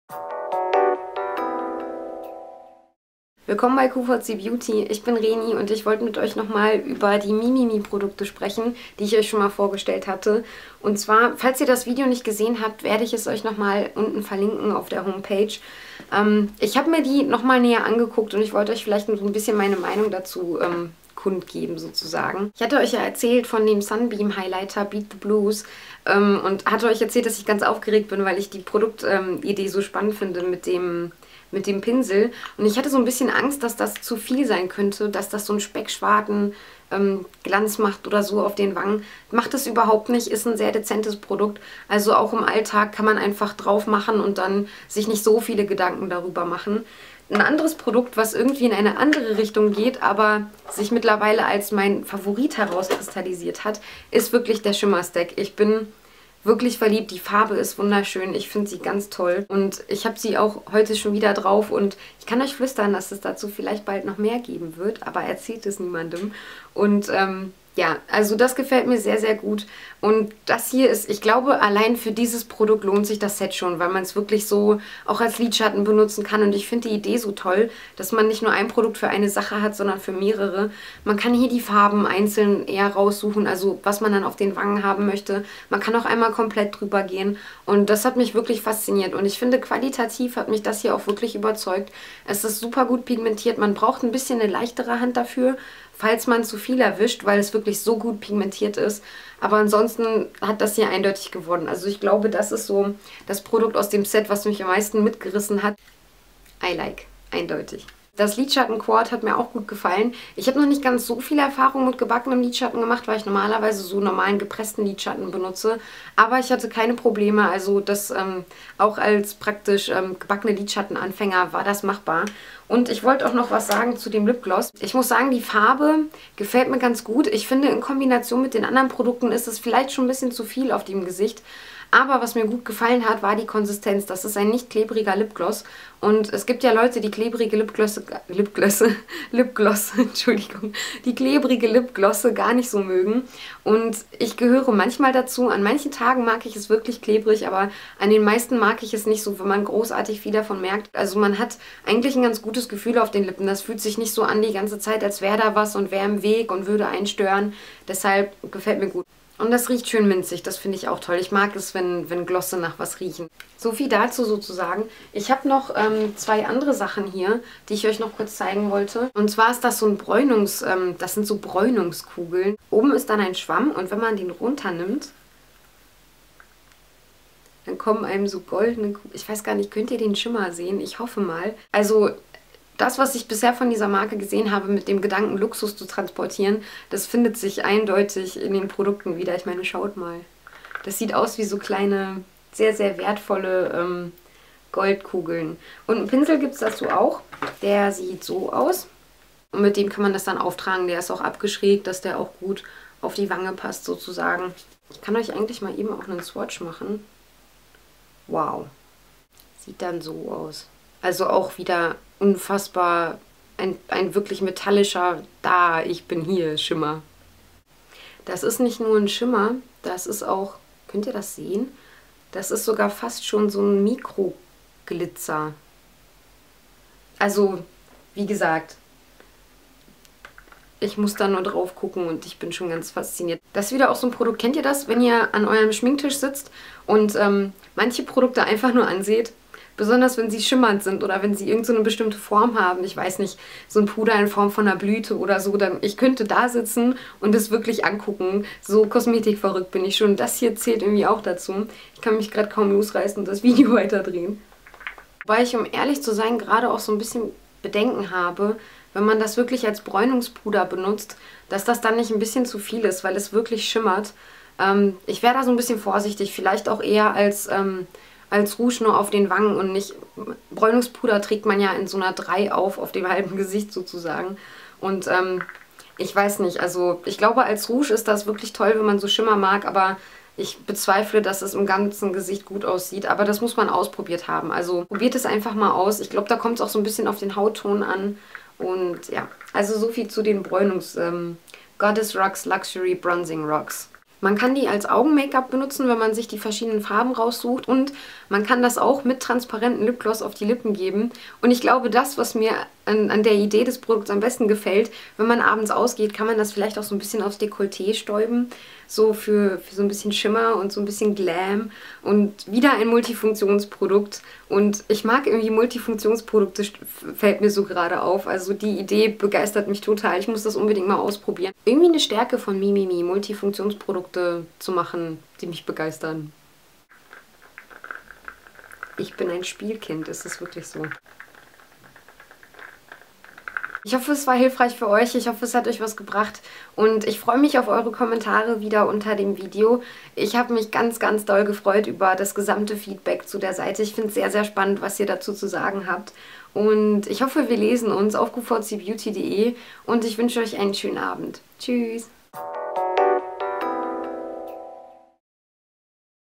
MeMeMe Goddess Rocks vorgestellt
Ich lasse heute die Goddess Collection noch einmal Revue passieren und werfe einen weiteren Blick auf die Goddess Rocks von MeMeMe.